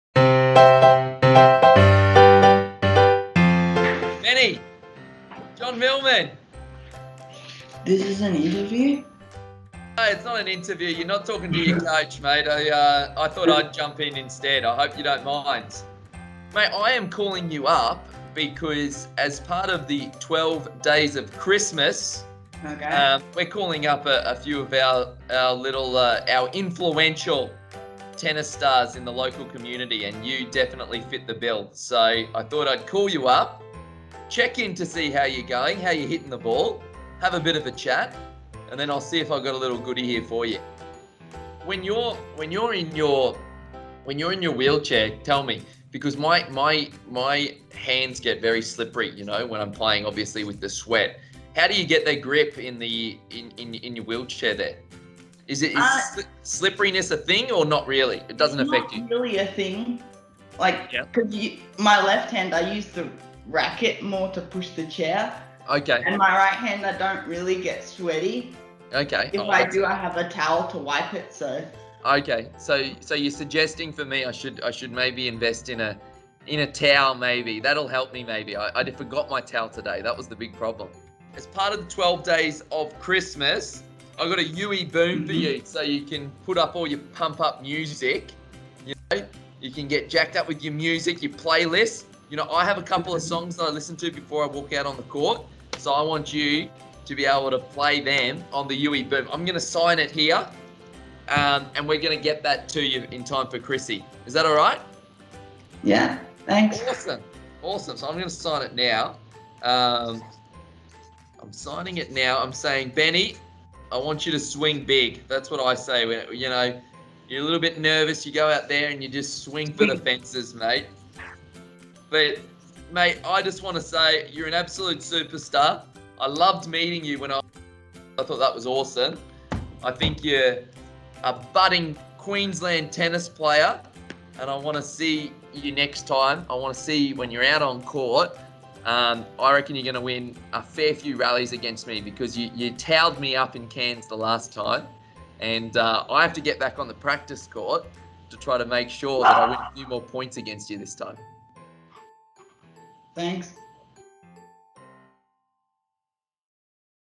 Day 2 of the 12 Days of Christmas
Day 2 of Tennis Queensland's 12 Days of Christmas and we have Australian Champion, John Millman, celebrating Ben Wenzel who continually encourages first timers and participation in wheelchair tennis

Keep up the good work Ben!

Start a conversation today to celebrate the achievements and contributions of the 4.4 million Australians, like Ben, with disability this International Day of People with Disabilities